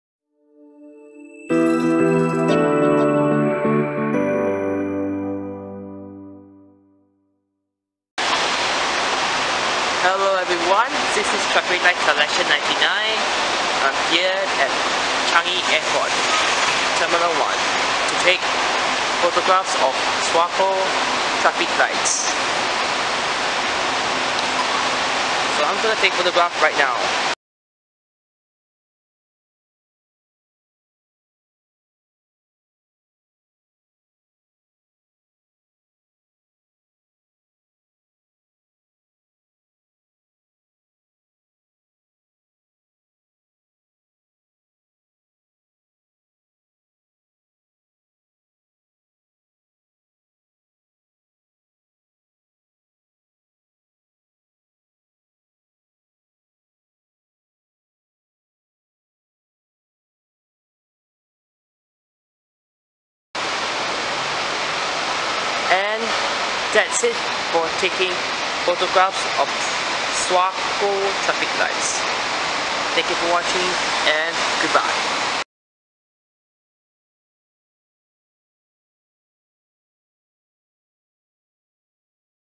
Changi Airport Terminal 1 Traffic lights (SWARCO Alustar) [Remastered]
This video is to take photographs of traffic lights at Changi Airport Terminal 1. Taken on 20 July 2018.